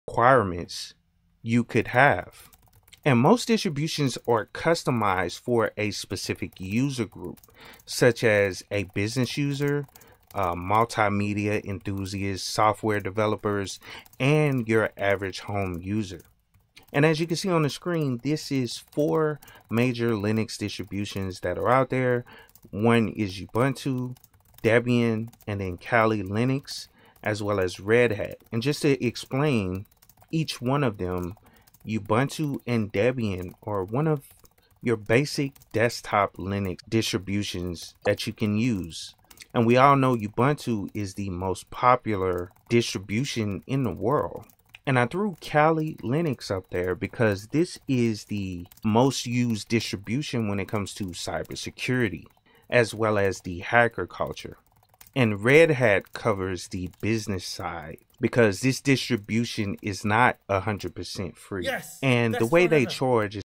118 words per minute, my best run ever.
I got to 118 words per minute on the typing test, which was faster than any of my runs before this.